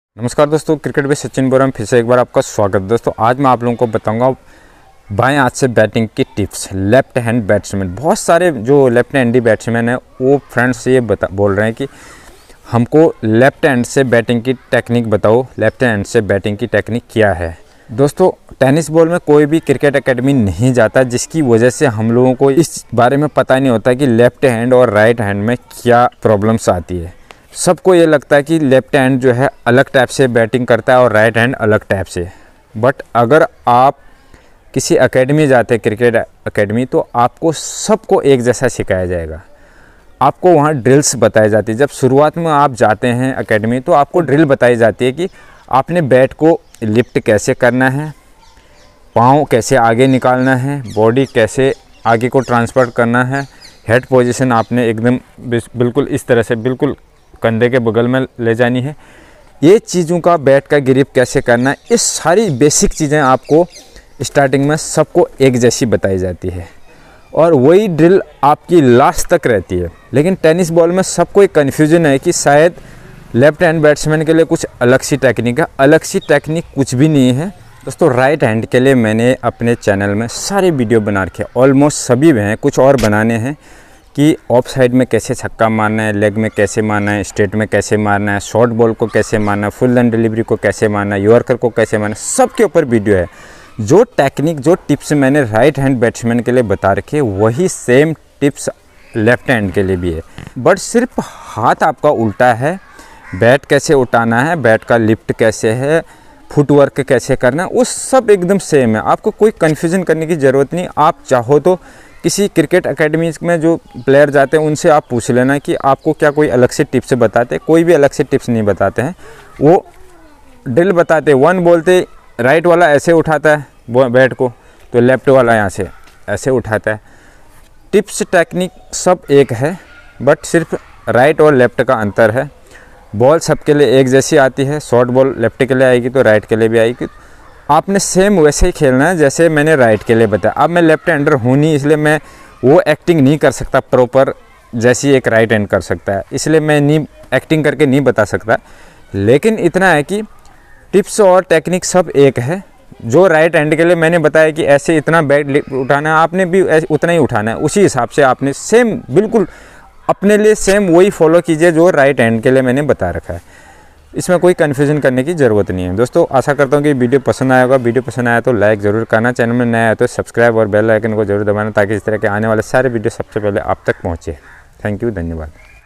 Tennis ball cricket left hand batting tips | left hand batsman kese batting kare
Welcome to cricket with Sachin Bora, in this video I explained about batting tips of left hand batsman, many friends asked me difference between left hand batsman and right hand batsman in tennis ball cricket, I cleared doubt in this video.